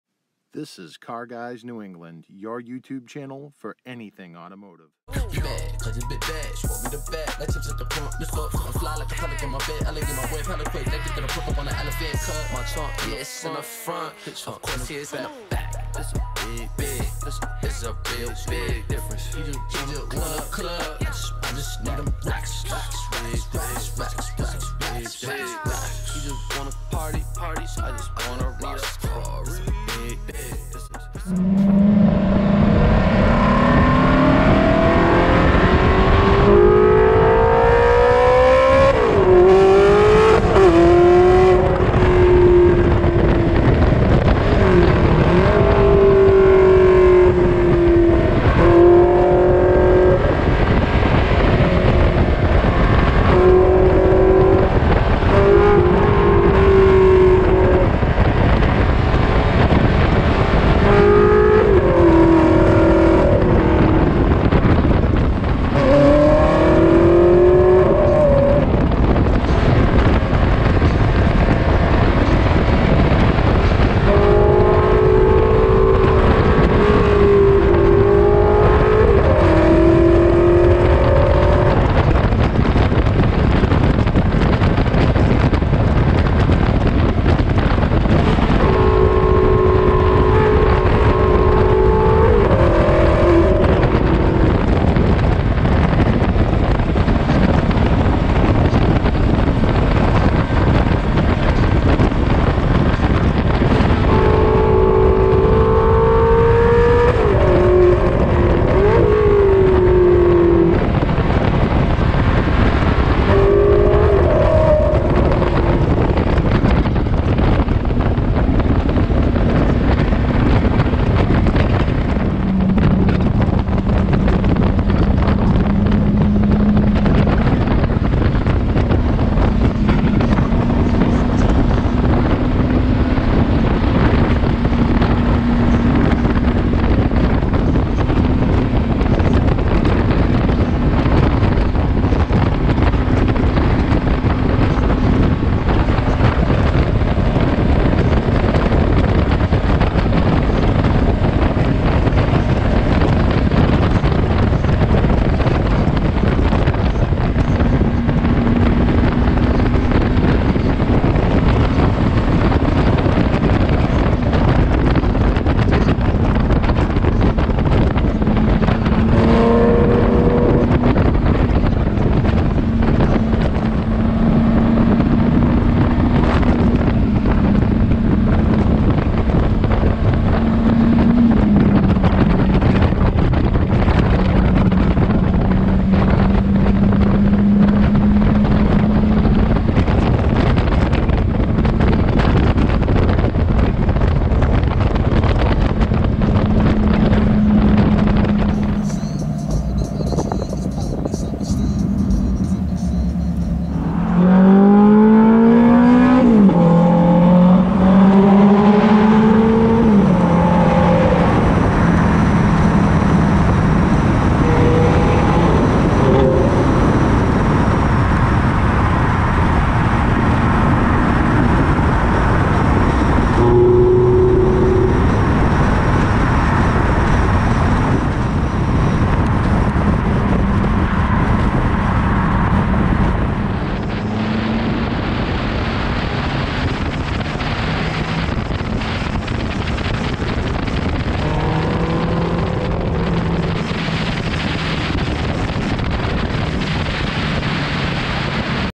Ferrari F430 Spider Ride
Going for a ride in the Ferrari F430 Spider with the GoPro Hero 7 Black.